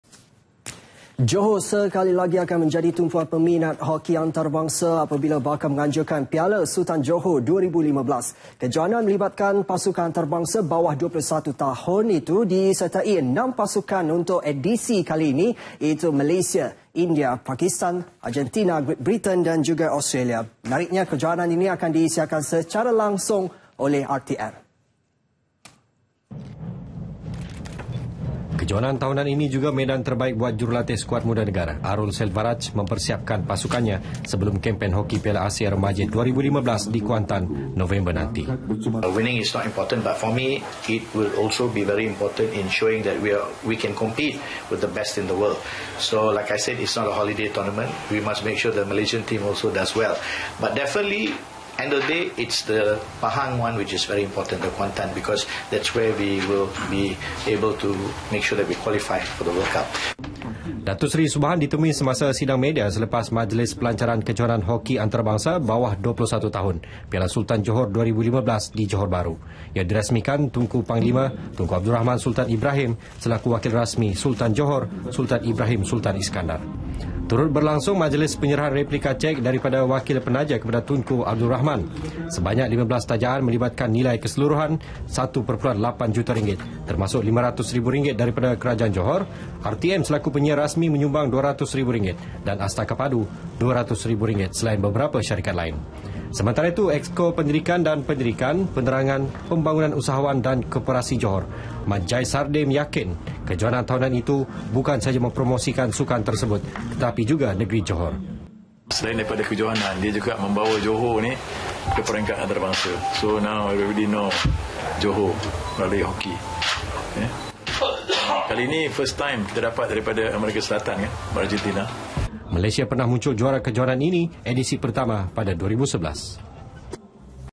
HOKI PIALA SULTAN JOHOR 2015 - 6 NEGARA AMBIL BAHAGIAN REBUT KEJUARAAN [8 SEPT 2015]
Johor sekali lagi akan menjadi tumpuan peminat hoki antarabangsa apabila bakal menganjurkan Piala Sultan Johor 2015.

Kejohanan melibatkan pasukan antarabangsa bawah 21 tahun itu disertai 6 pasukan untuk edisi kali ini iaitu Malaysia, India, Pakistan, Argentina, Great Britain dan Australia.

Menariknya, kejohanan ini akan disiarkan secara langsung oleh RTM.